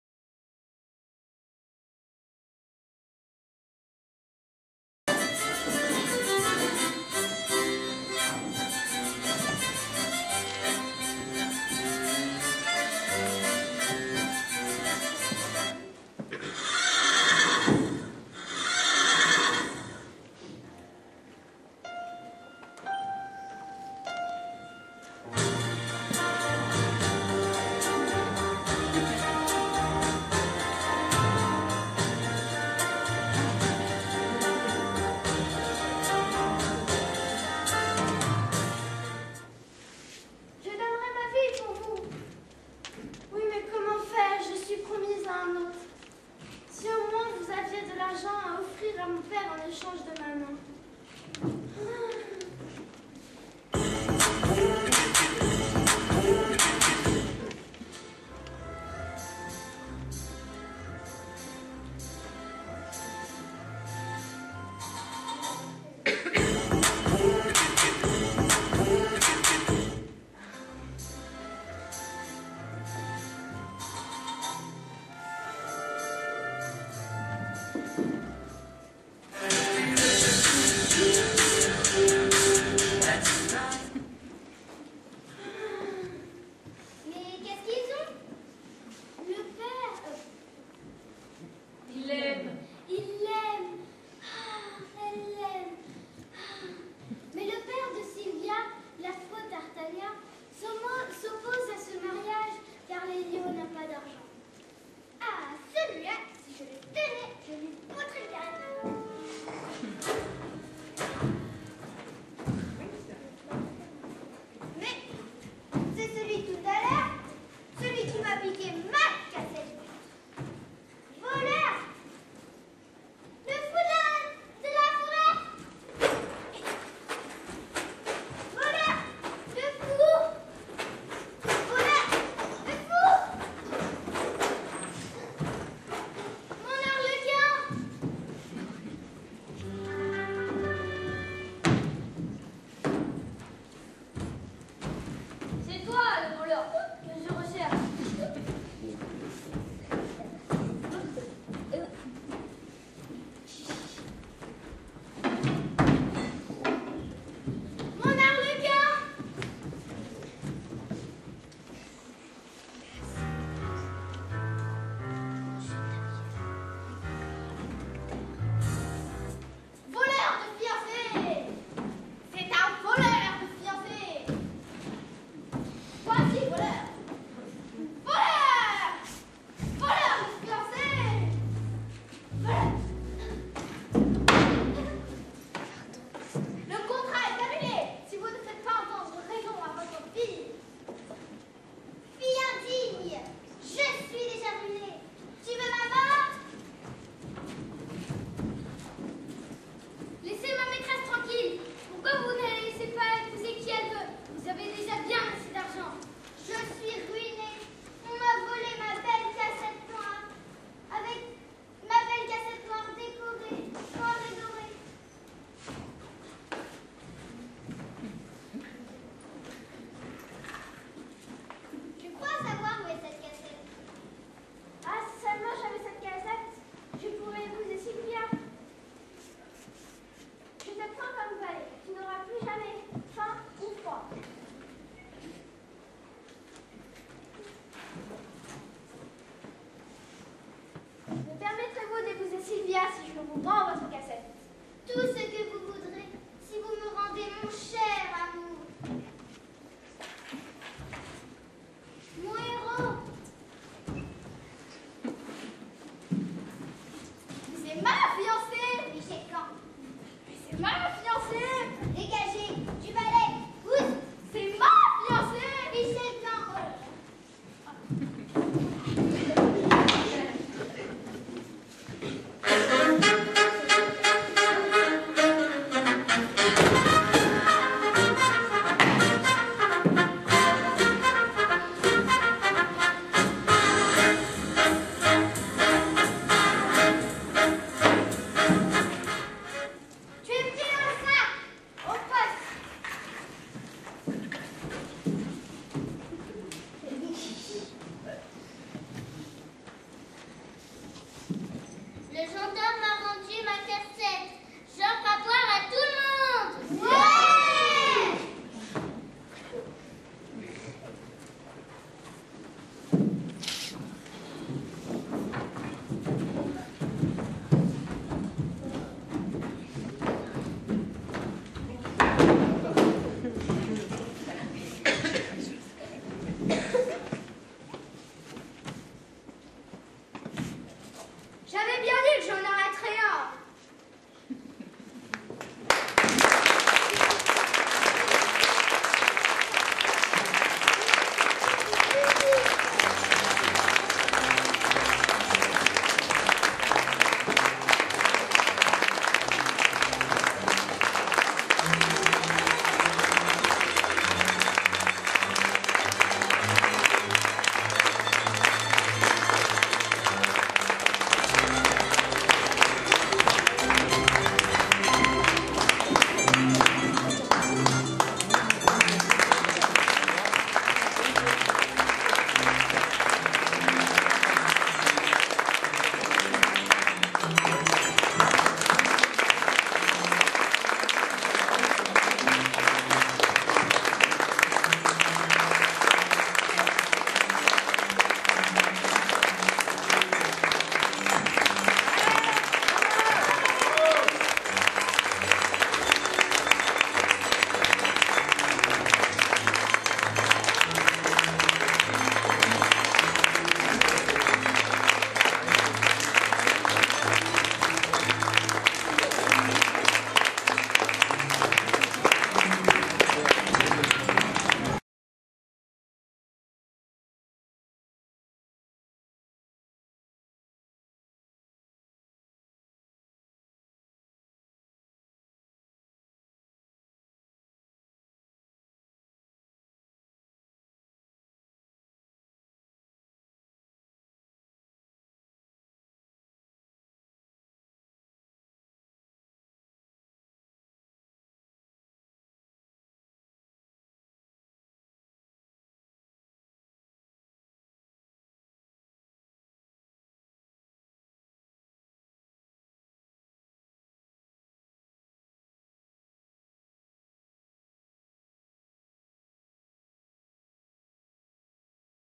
Une aventure d'Arlequin - le film - Acte III
Représentations de fin d'année des ateliers de l'ACAP - Saison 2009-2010
Atelier des 9-11 ans.
De et mis sn scène par Florence Murphy
Captation : Joanna Le Roy